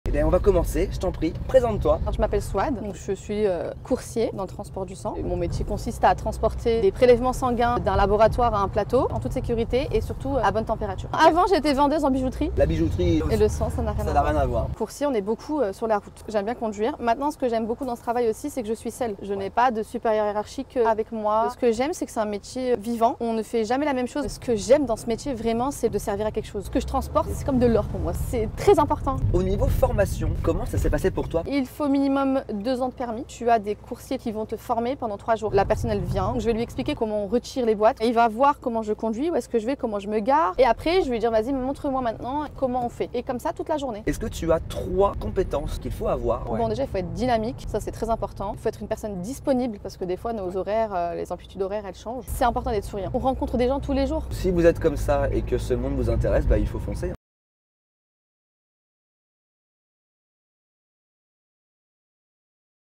Semaine du Transport et de la Logistique 2024 : « 1h de route avec un coursière »
À l'occasion de la Semaine du Transport et de la Logistique, France Travail a rencontré divers professionnels du secteur.

Découvrez dans cette vidéo le témoignage d'une coursière.